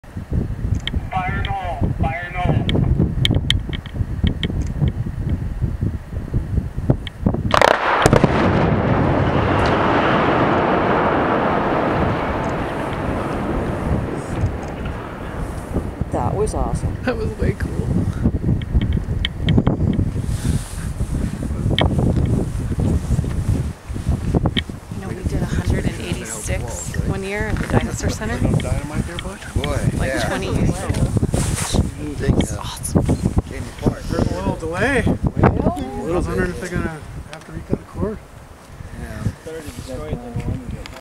Wind River Canyon Blast
The Wyoming Department of Transportation closed Wind River Canyon this morning in order to blast out a rock that was presenting a potential danger to drivers if it came loose. The blast was successful and the road was opened within an hour.

Videographer - Cindy Glasson, Thermopolis Independent Record